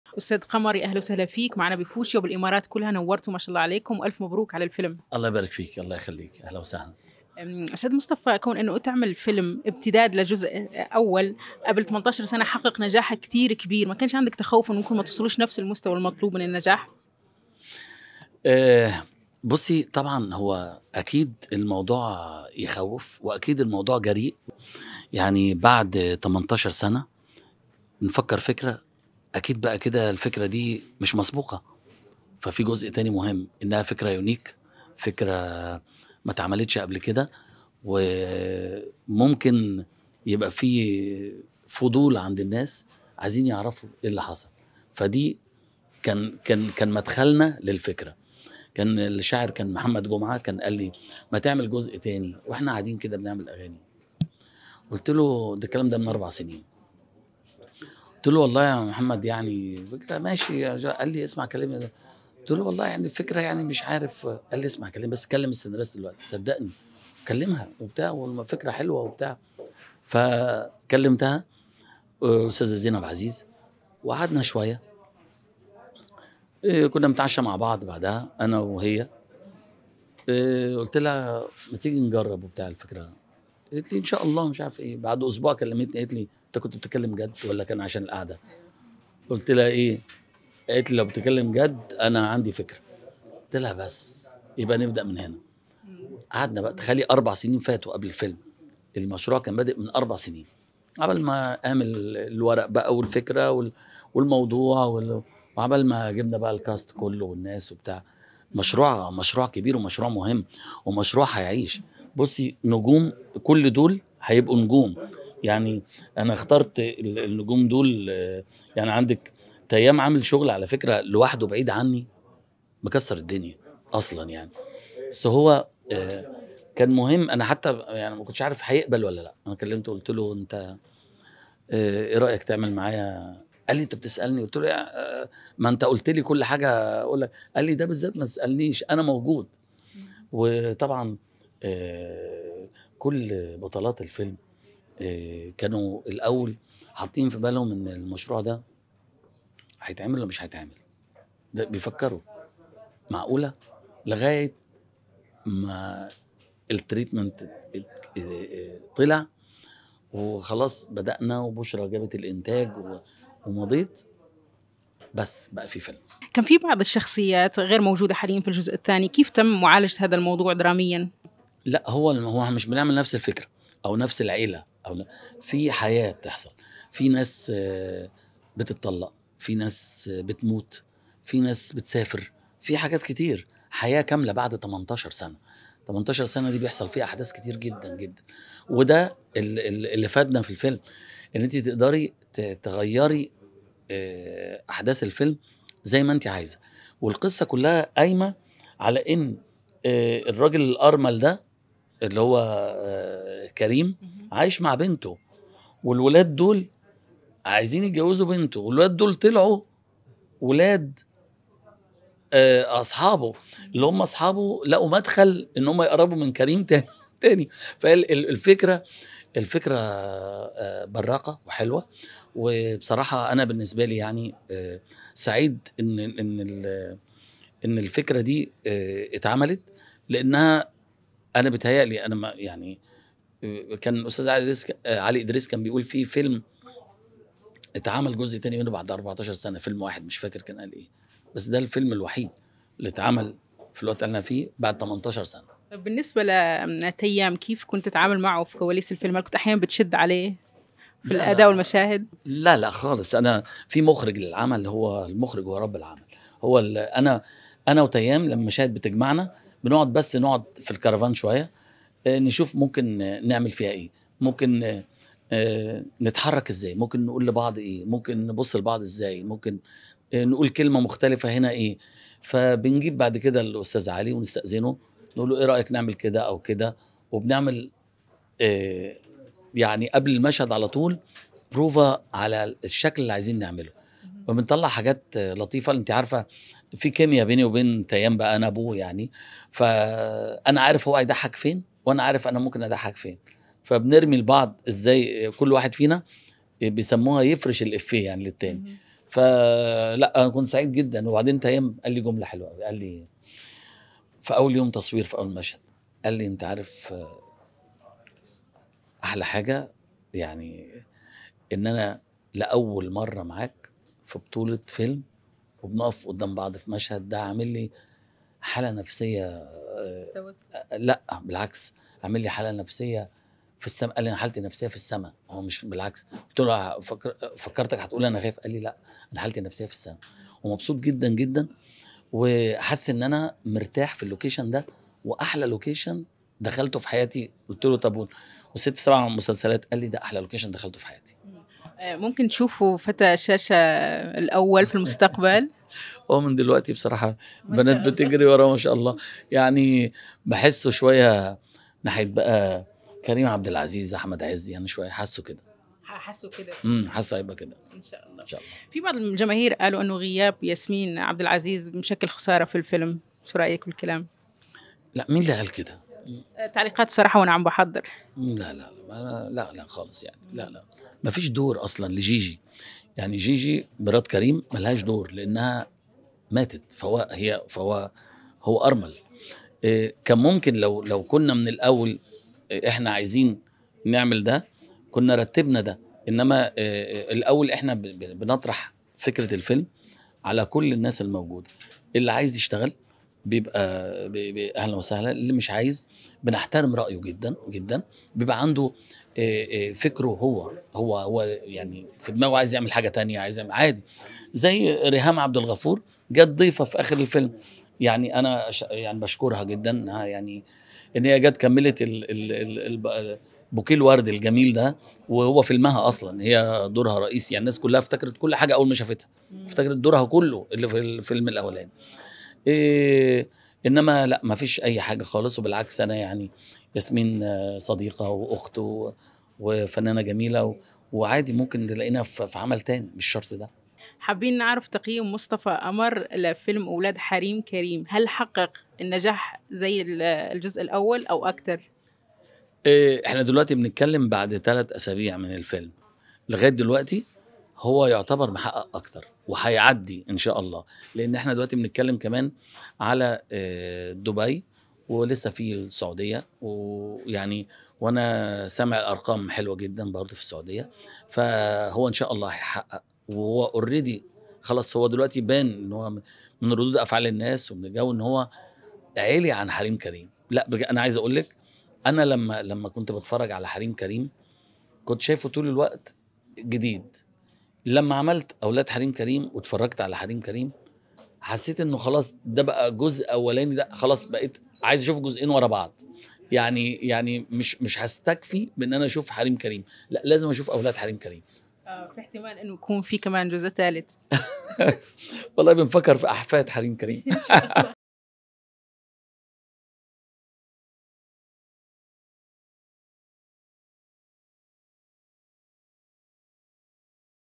مصطفى قمر: ابني يشبه أحمد عز وكريم عبدالعزيز
في تطور غير متوقع في عالم السينما المصرية، قام الفنان قمر مصطفى بتقديم جزء ثانٍ من فيلمه الشهير "حريم كريم" بعد مرور 18 عاماً على إصدار الجزء الأصلي.